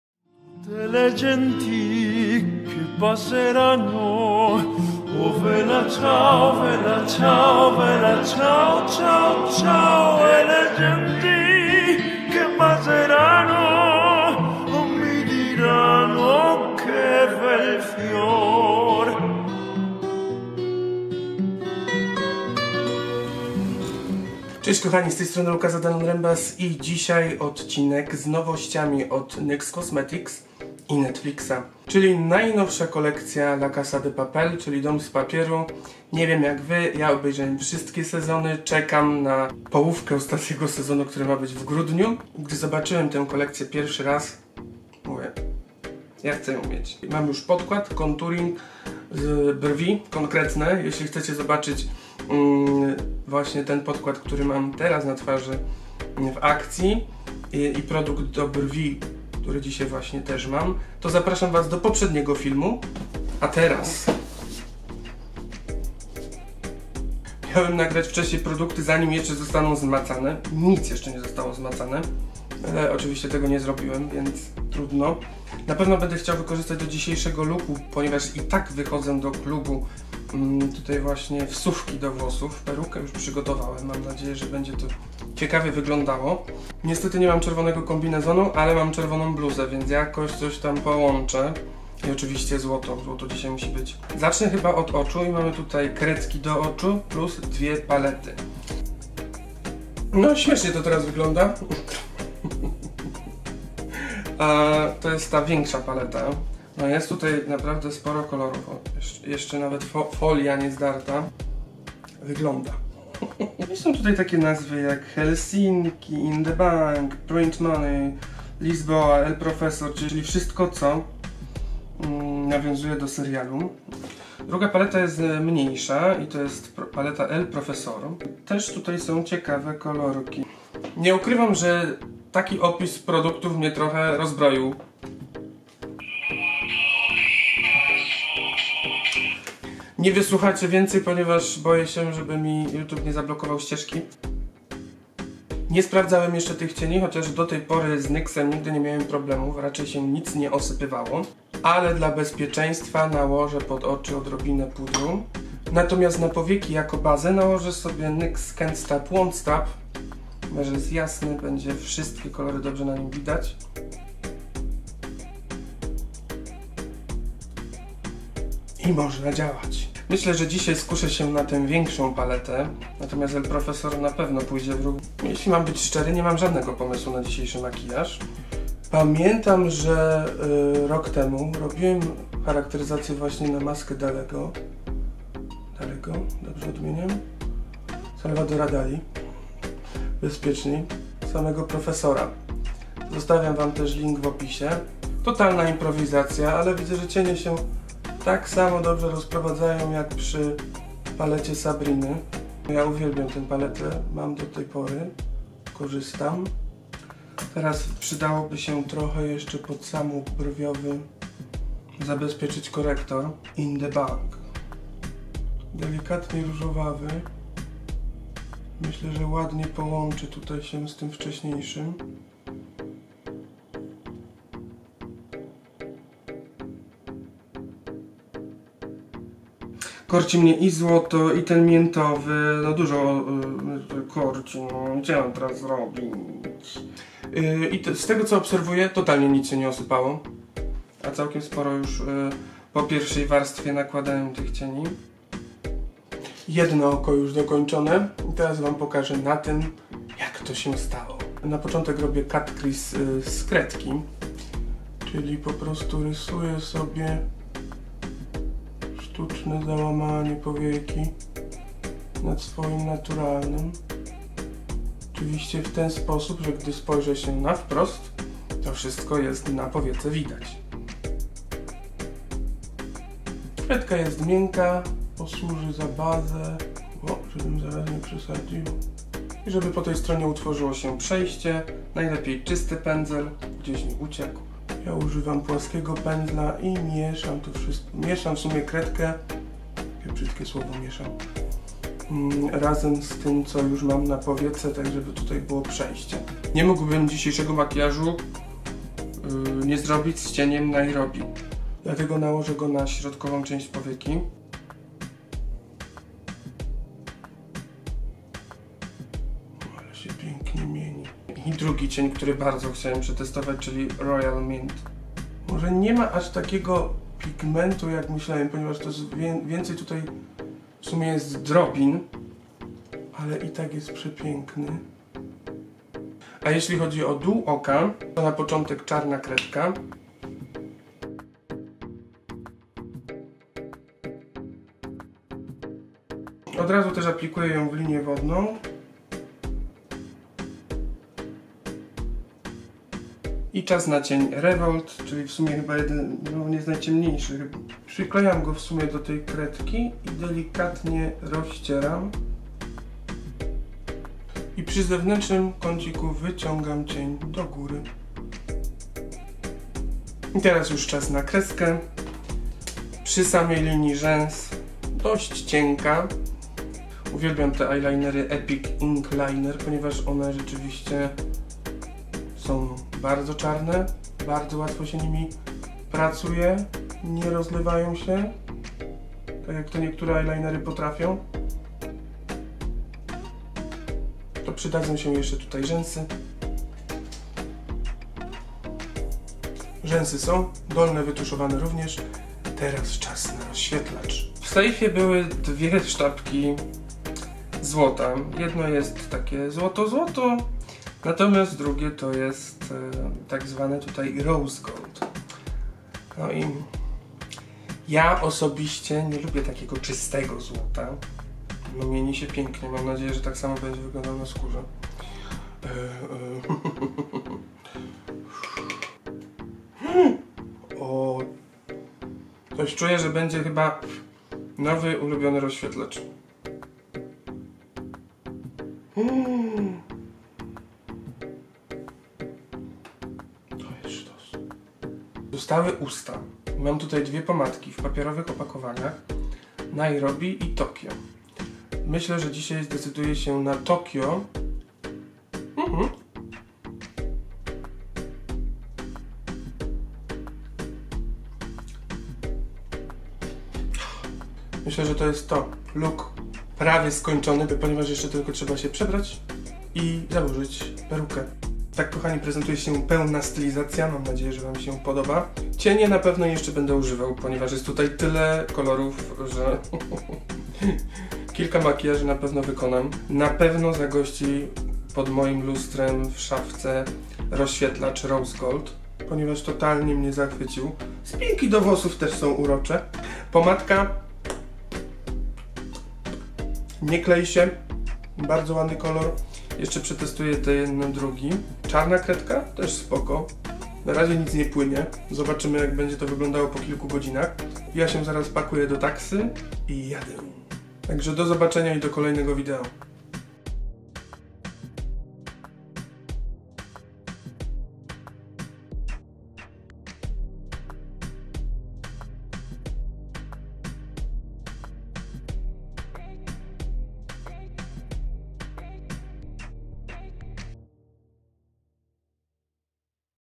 NYX Cosmetics x La Casa De Papel | Kolekcja kosmetyków 💄💰
Charakteryzacja Maska i Profesor z serialu "Dom z papieru"